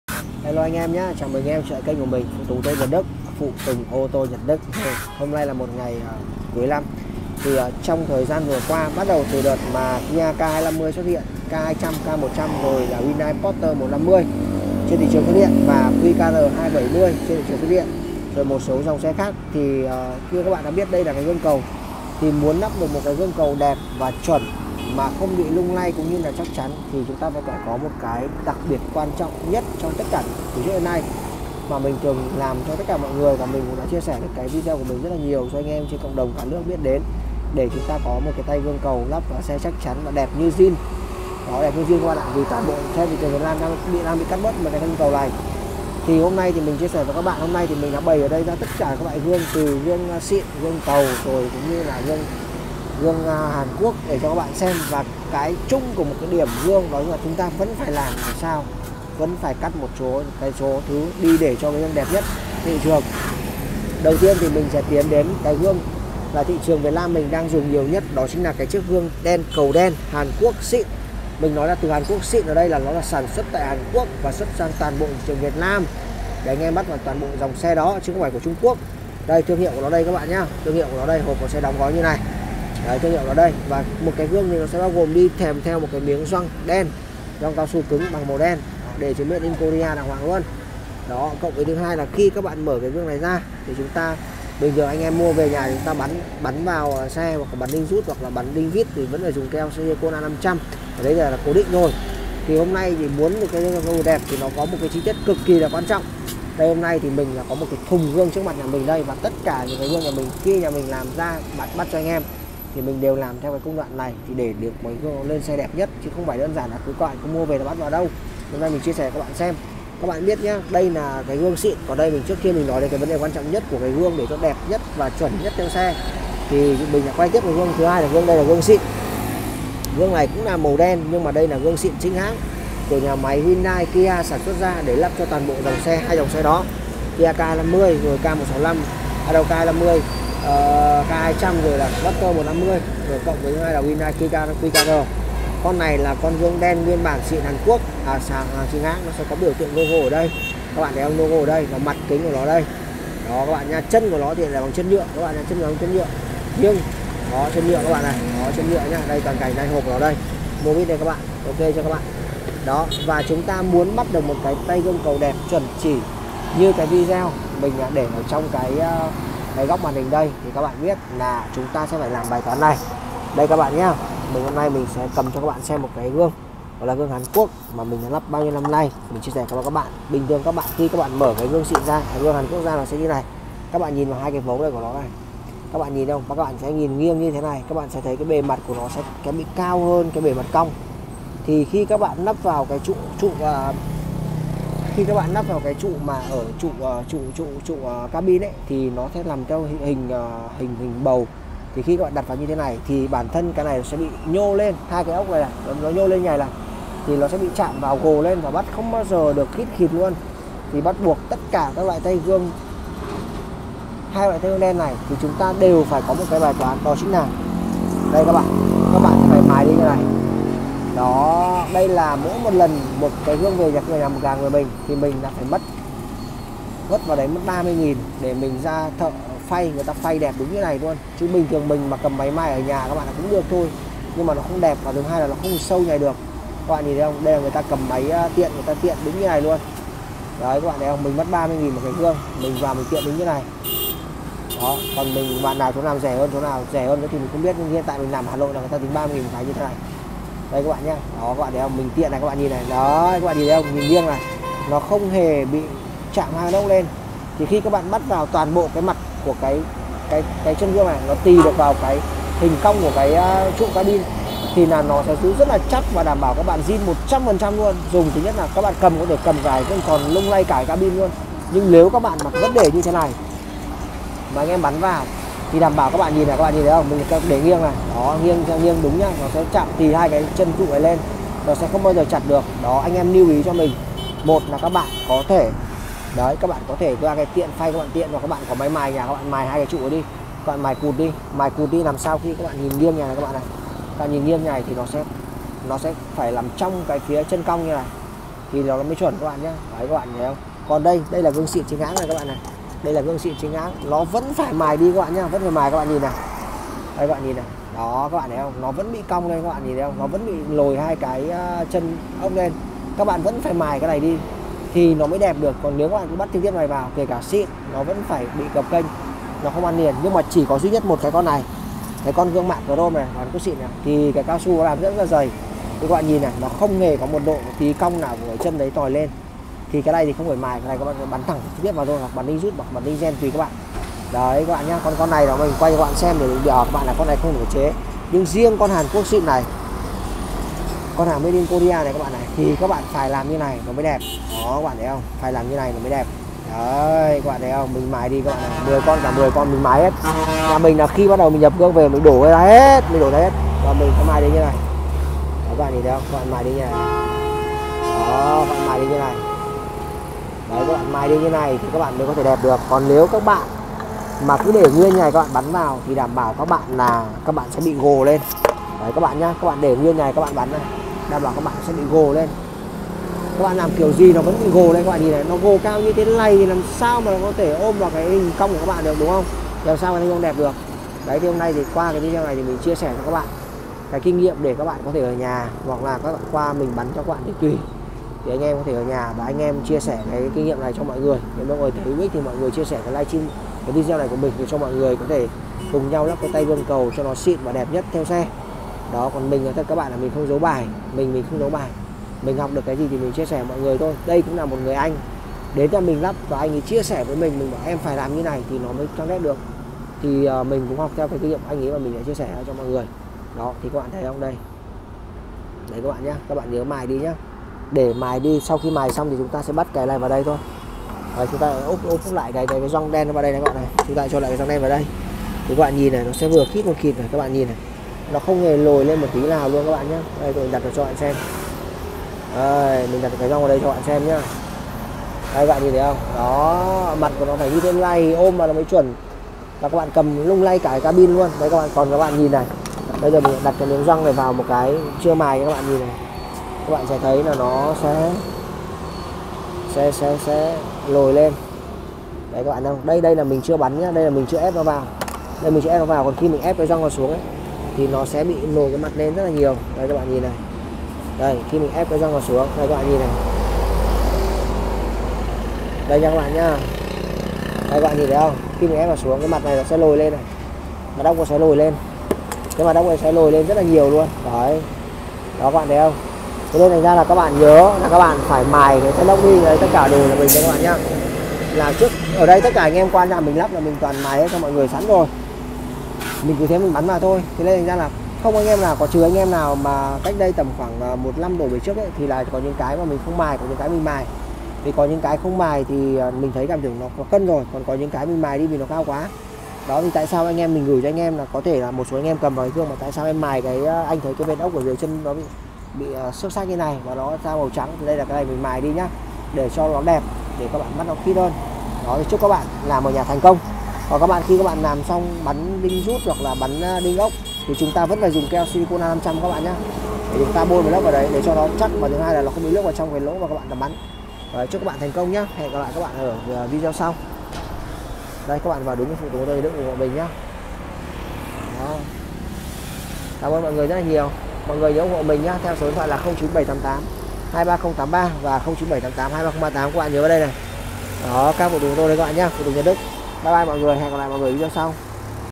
Review chia sẻ kinh nghiệm lắp tay gương tròn bắt vừa khít và chắc như Đinh thì nhất định phải cắt
☎các bác cần gì cứ alo 24/24 H